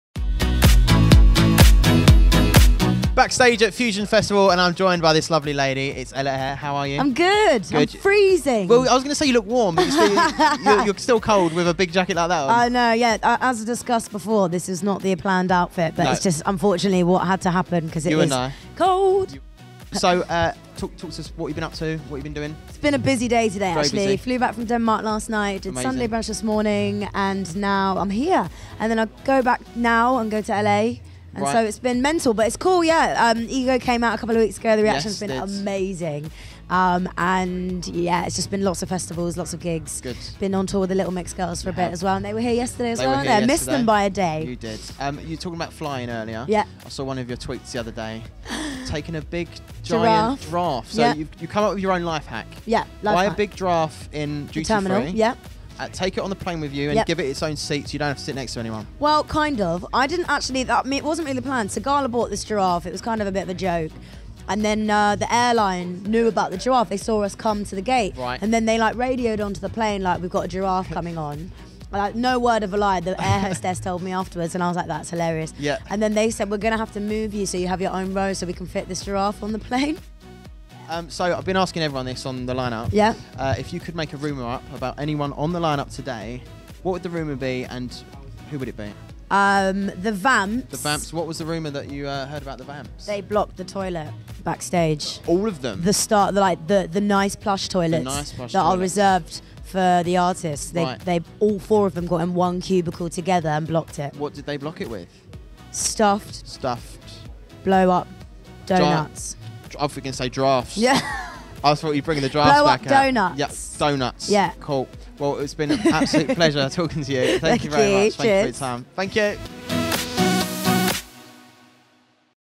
The official Ella Eyre interview at Fusion Festival 2017
Sonny Jay interviews Ella Eyre about recent activities, airport lifehacks with giraffes and more in this official Fusion Festival interview.